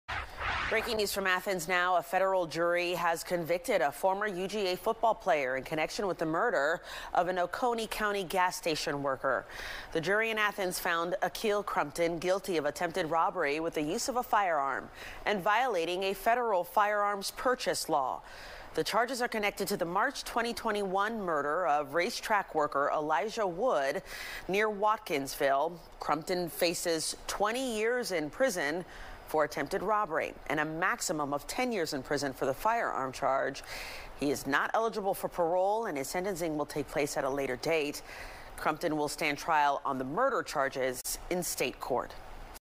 Former UGA football player charged in connection with murder | FOX 5 News
A federal jury has convicted Akhil Crumpton, a former UGA football player, in connection with the murder of an Oconee County gas station worker. The jury found him guilty of attempted robbery with the use of a firearm and violating a federal firearms purchase law. The charges are connected to the March 2021 murder of Racetrac worker Elijah Wood.